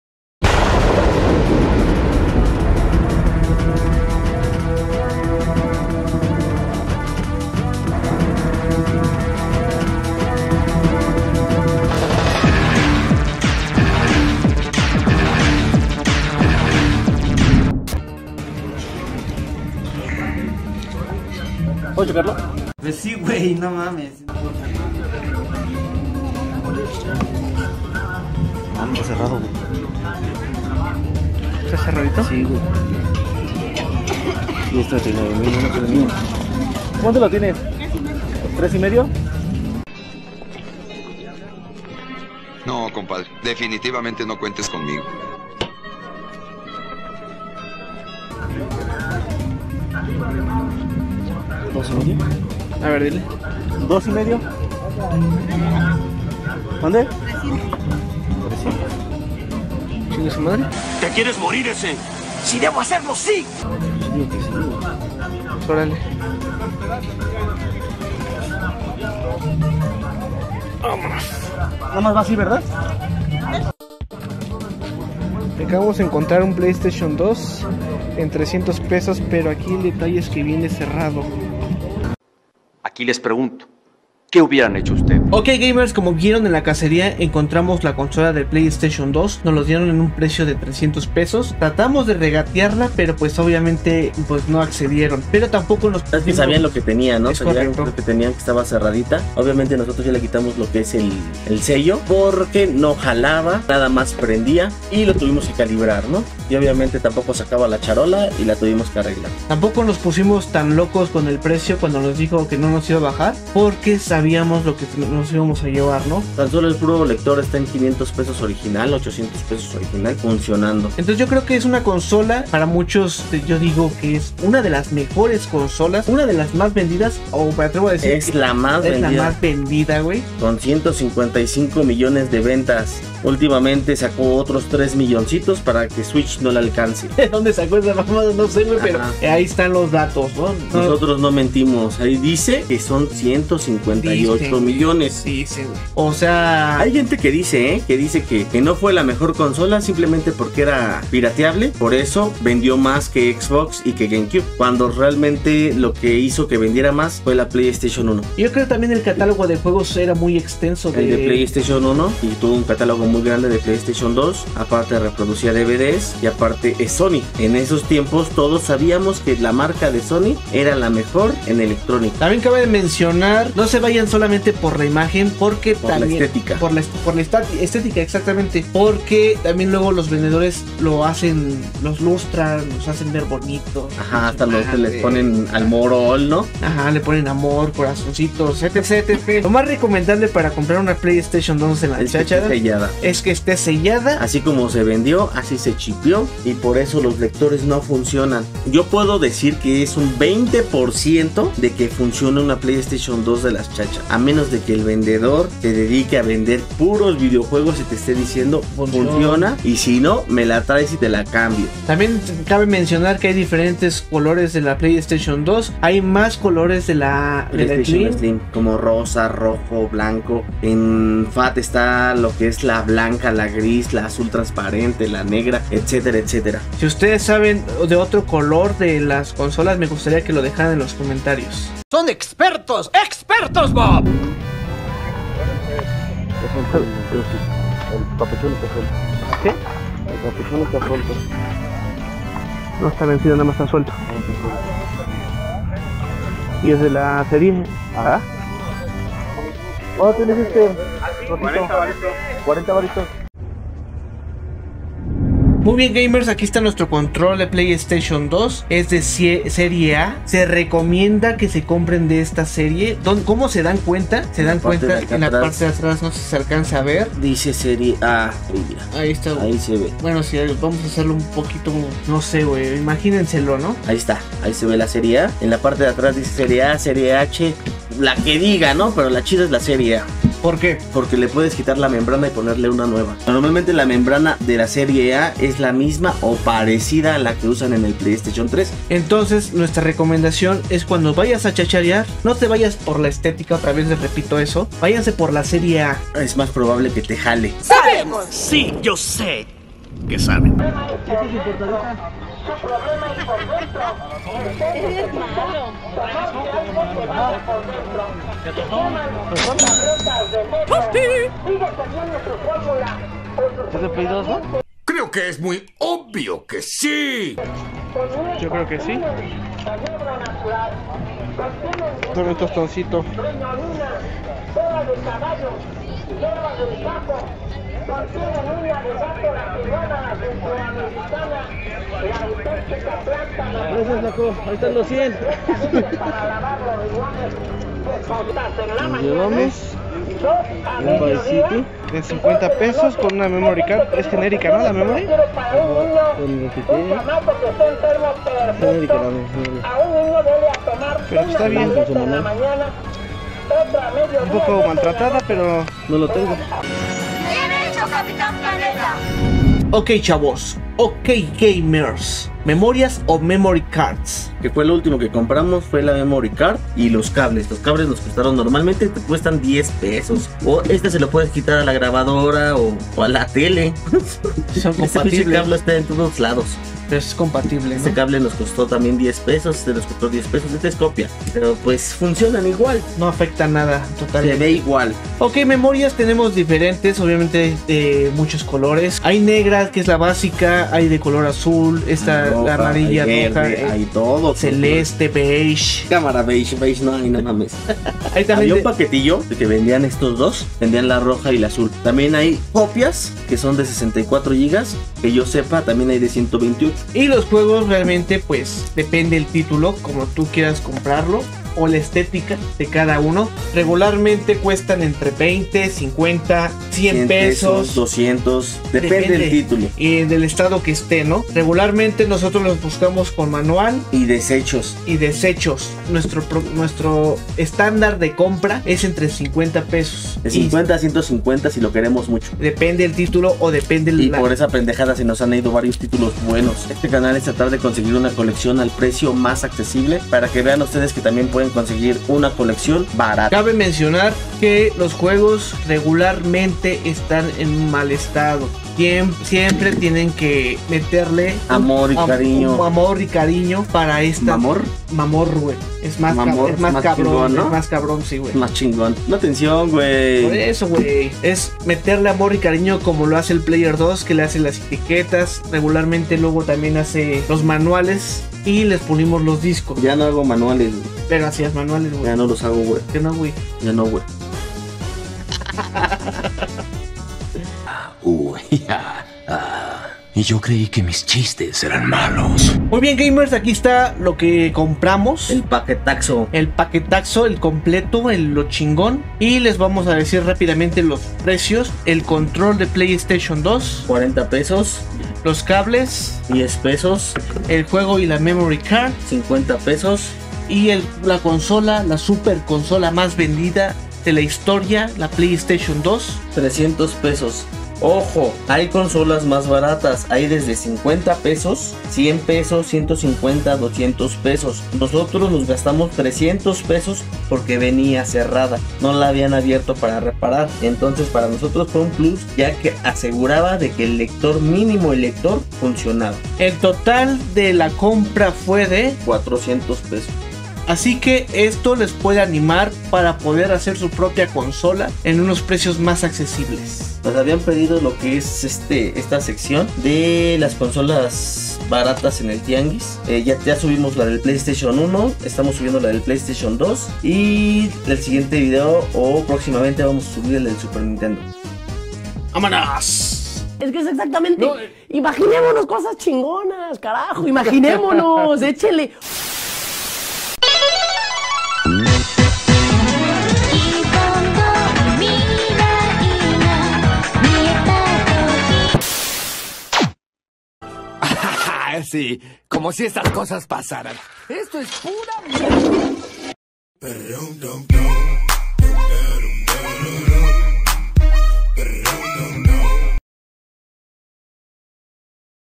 La Ps2 mas barata que hemos comprado ¡Imperdibles videojuegos a precio de ganga en tianguis!
Buscando consolas y videojuegos en los tianguis de chacharas
Comprando videojuegos para la venta
formando la colleccion a un precio accesible
todo lo referente a videojugos es importante para nosotros
en esta ocacion la tematica es de chip and dale